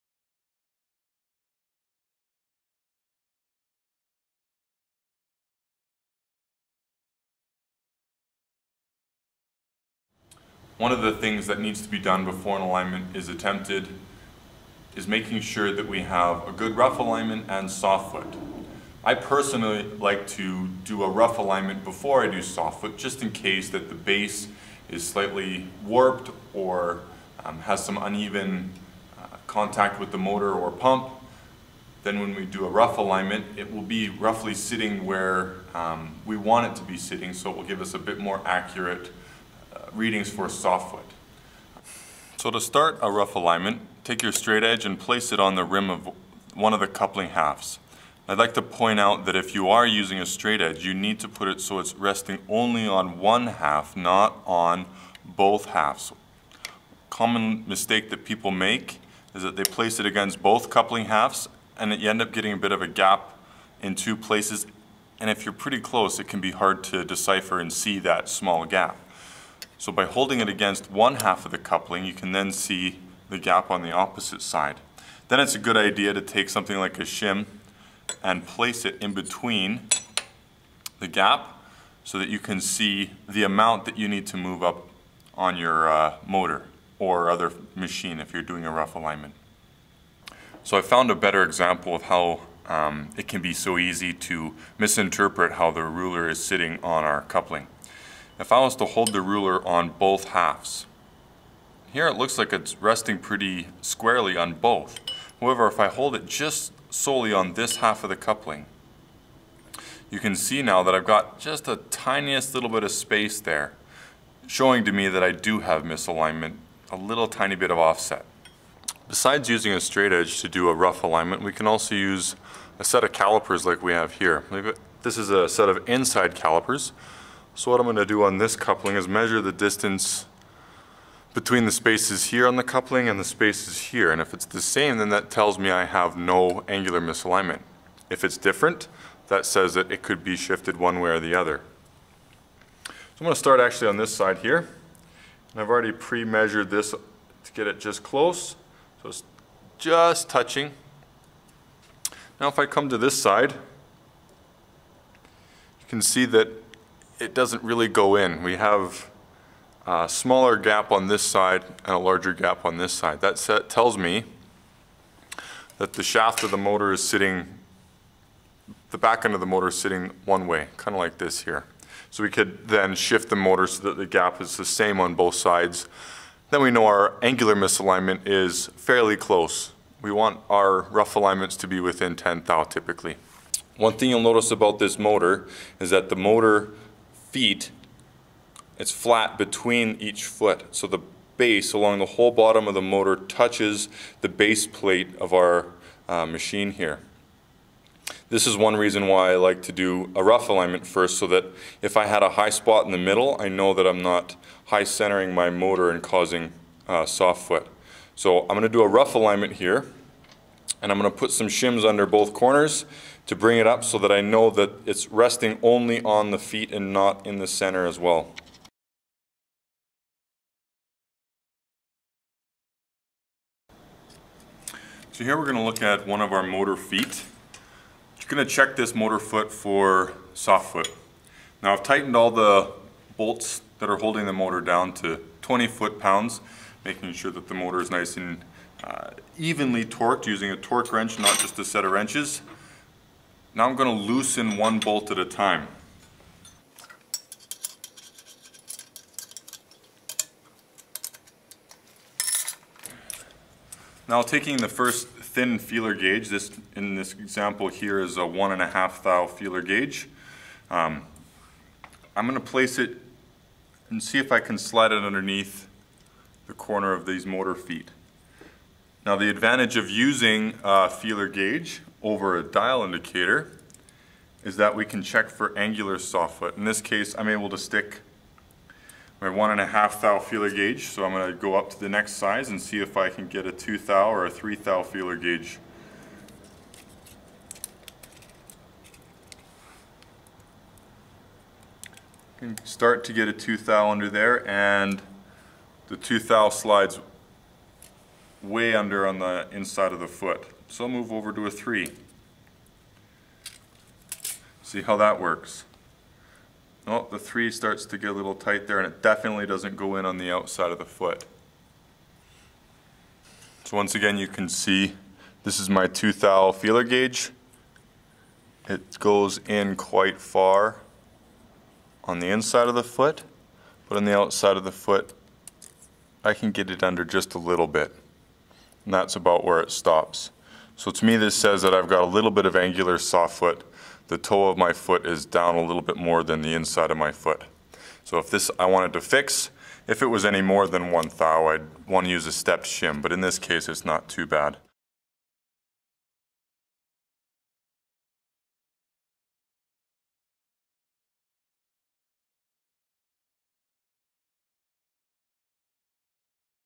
Soft foot and rough shaft alignment
How to check for and correct soft foot. How to perform a rough alignment.